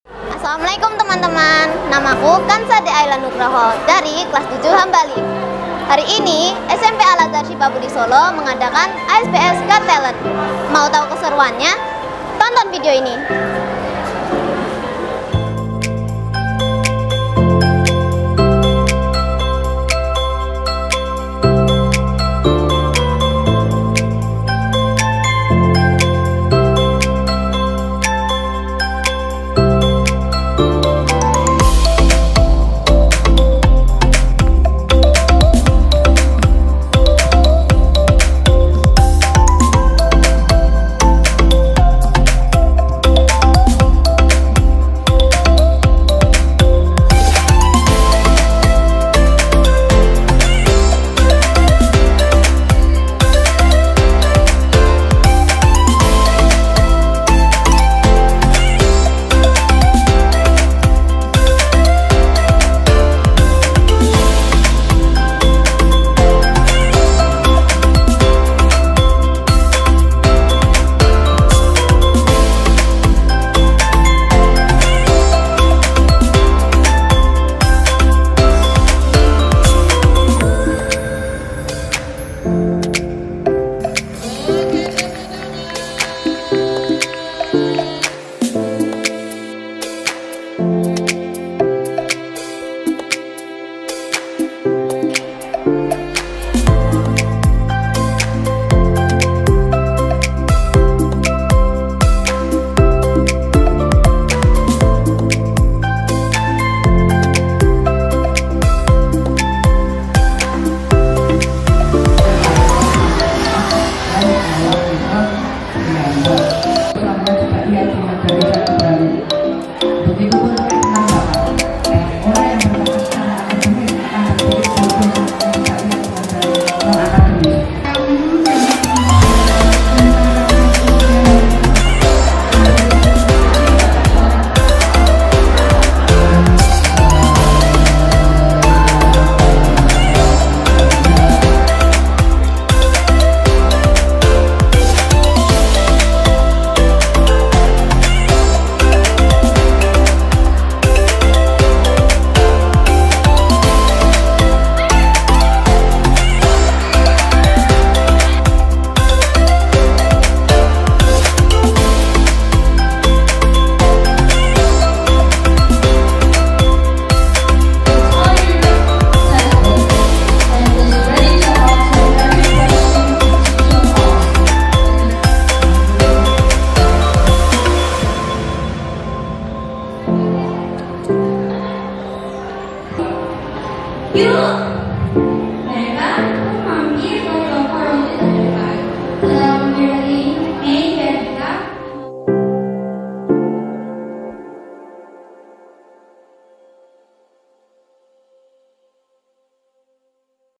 ASBS GOT TALENT 2023 | SMP AL-AZHAR SYIFA BUDI SOLO
ASBS UPDATE

Wah temen-temen dari kelas 7 SMP Al-Azhar Syifa Budi Solo keren keren semua nih, punya bakat dan potensi yang luar biasa 🤩

Nah tentunya minat & bakat dalam ajang ASBS GOT TALENT ini akan dikembangkan bersama Yanda Bunda di sekolah yaa...

"Membangun Inovasi Pendidikan Berkelanjutan" 📈

Al-Azhar Syifa Budi Solo
Islamic School of Excellence
Mumtaz School
Daftar Online 👉ppdb.asbsolo.sch.id